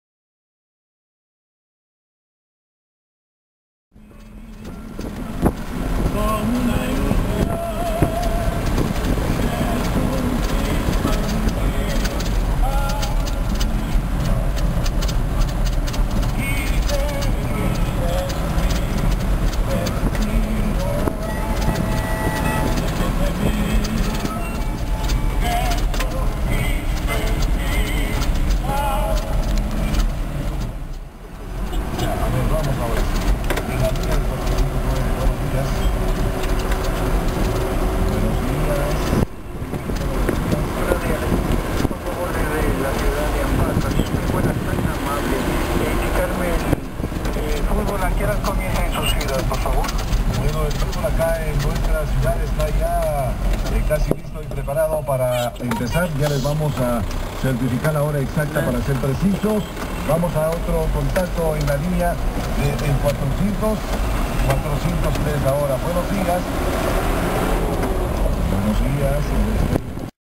Montañas de Alausí - Ñariñachi
Todavia existen lugares en el Ecuador que no están acorde con el nuevo siglo